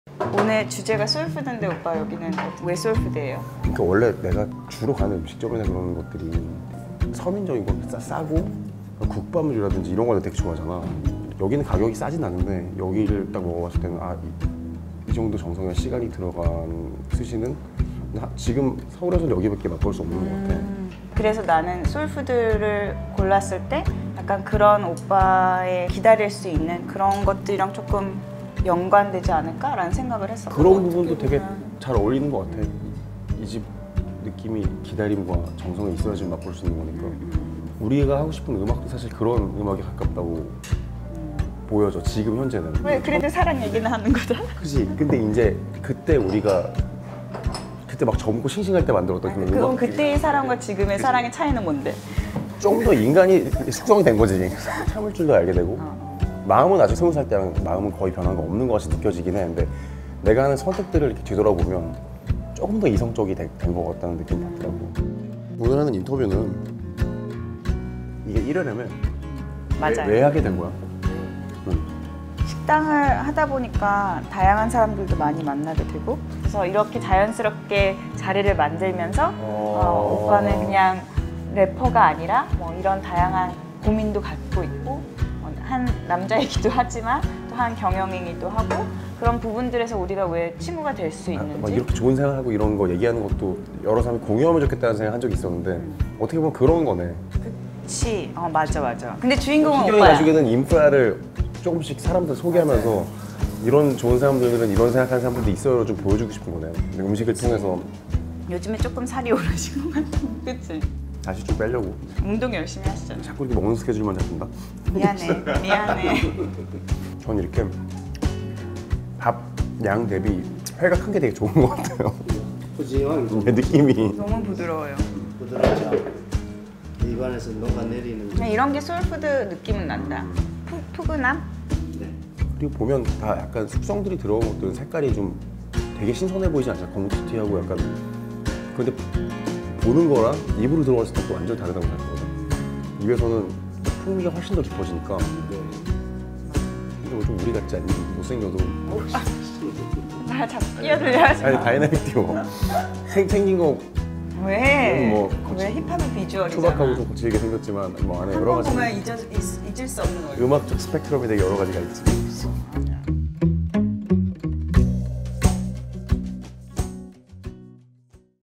Life Recipe #1 최자의 소울 푸드, 타구미곤 _ 마리끌레르 라이프 레서피
레스토랑 ‘가온’, ‘비채나’의 조희경 대표와
다이나믹 듀오의 최자가 식사를 함께 했어요.
의외의 조합이다 싶은 두 사람은 알고 보니 오랜 친구 사이.
올해에도 미쉐린 4스타를 획득한 조 대표, 최자 또한 음식 사랑으로 아주 유명하죠?
음식이라는 공토분모를 가진 두 사람이 레스토랑 ‘타쿠미곤’에서 만났습니다.
타쿠미곤은 권오준 셰프의 장인 정신이 담긴 숙성 스시로 이름난 곳이죠.
음식뿐 아니라 랩, 사업에 대해 나눈 그들의 이야기를 만나보세요!